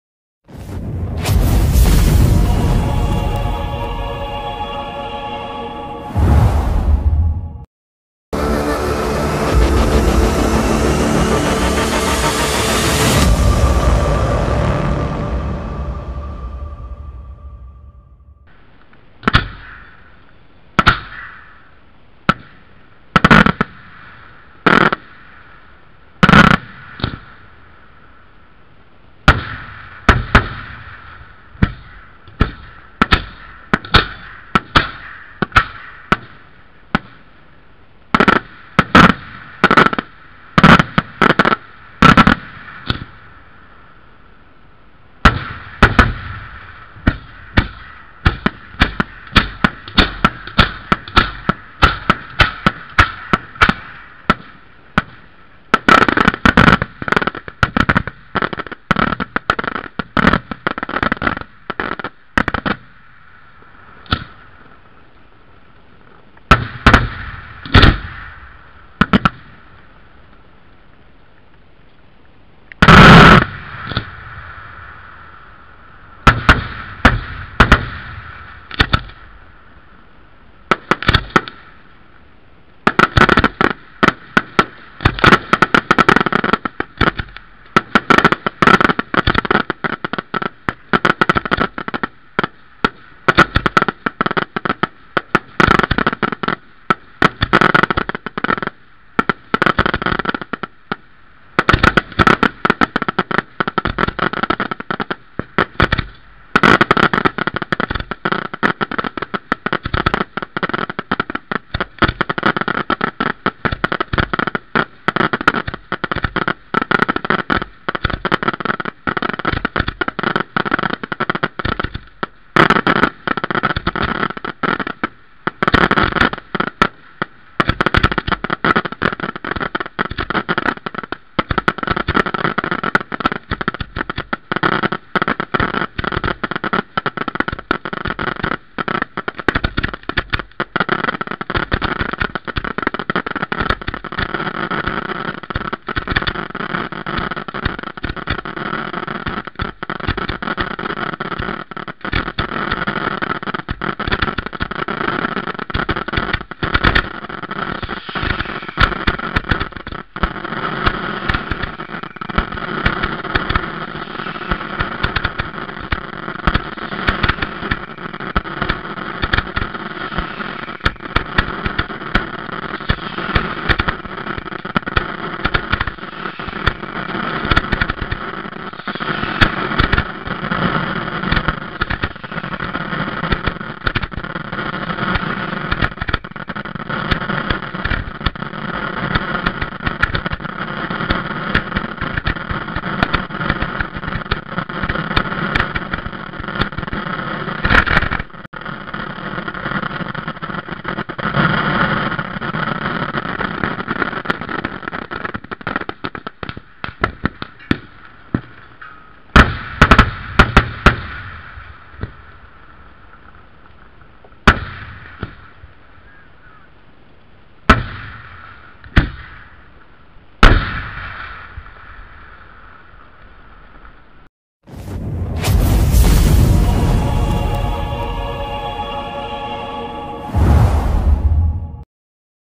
Adelfia (Ba) - San Trifone 2021 - Danger Cam - Scarica Palmieri De Candia
DangerCam sulla scarica dello spettacolo pirotecnico aero diurno eseguito dalla ditta Palmieri & De Candia Fireworks di Antonio Palmieri e Mino De Candia da Apricena (Fg).
Video by JM Editor